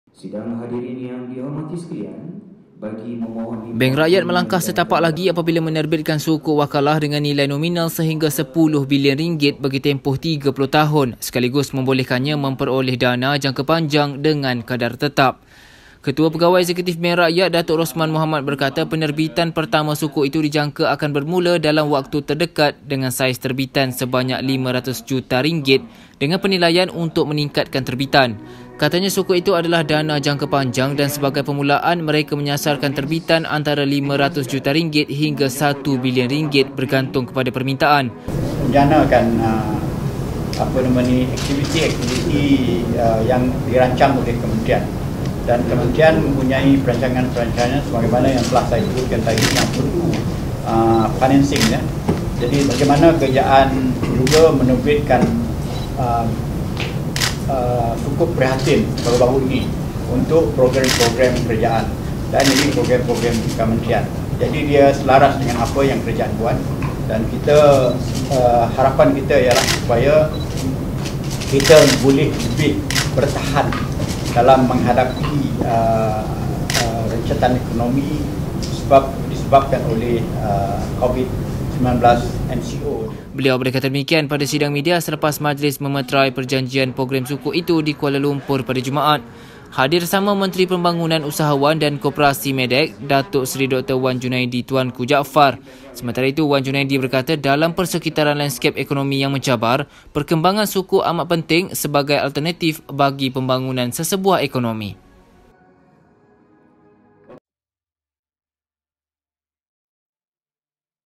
Bank Rakyat terbit Sukuk Wakalah RM10 bilion
KUALA LUMPUR: Bank Rakyat akan menerbitkan Sukuk Wakalah dengan nilai nominal sehingga RM10 bilion untuk tempoh 30 tahun bagi membolehkannya memperoleh dana jangka panjang dengan kadar tetap.